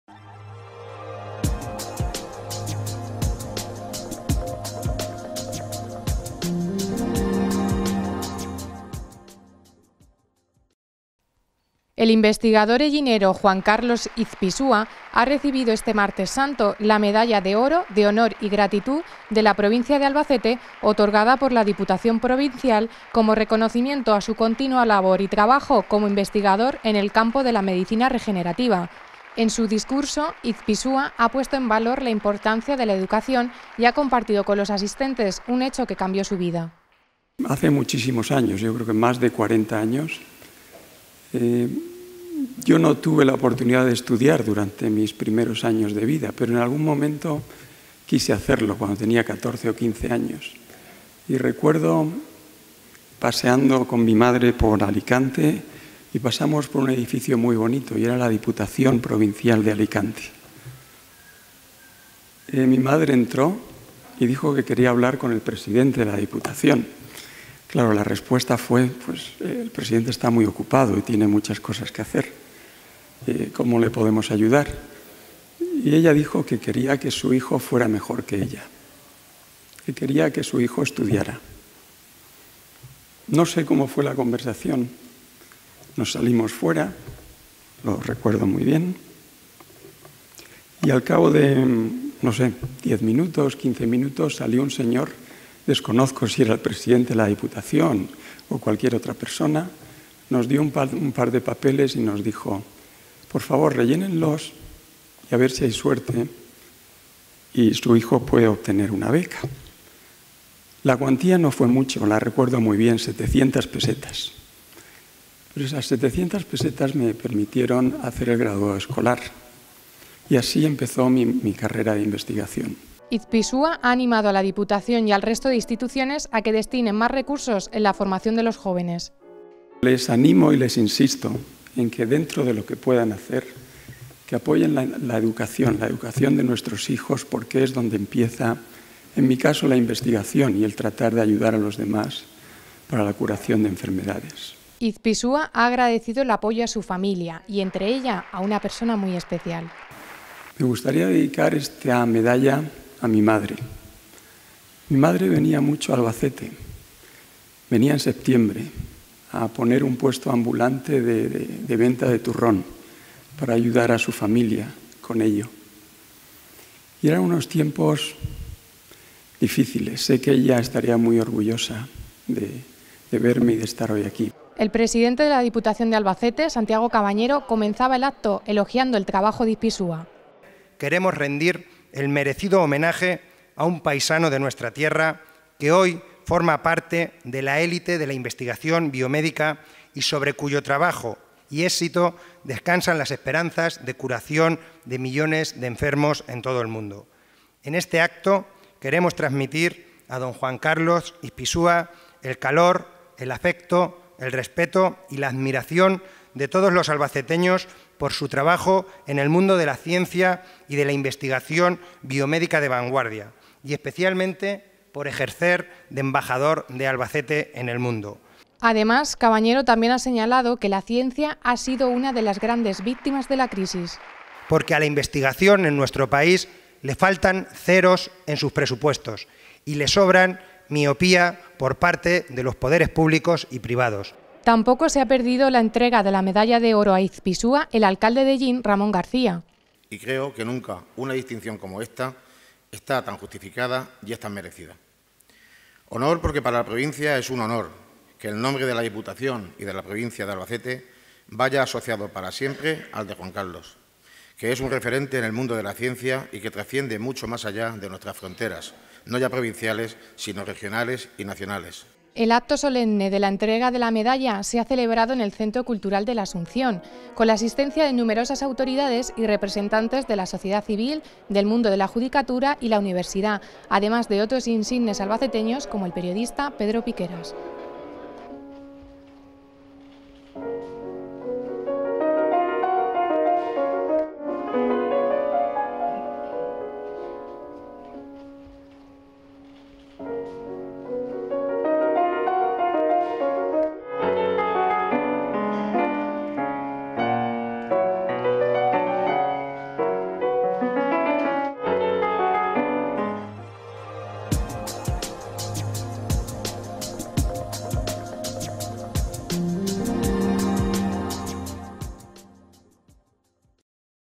Albacete rinde homenaje al genio Izpisúa
El presidente de la Diputación Provincial de Albacete, Santiago Cabañero, ha condecorado este martes con la Medalla de Oro, de Honor y Gratitud de la Provincia de Albacete al investigador hellinero Juan Carlos Izpisúa Belmonte. Este reconocimiento fue aprobado por unanimidad en el pleno de la Diputación en la sesión extraordinaria celebrada el pasado 10 de marzo a propuesta de los cuatro grupos políticos representados en la Diputación de Albacete como reconocimiento a su continua labor y trabajo como investigador en el campo de la medicina regenerativa.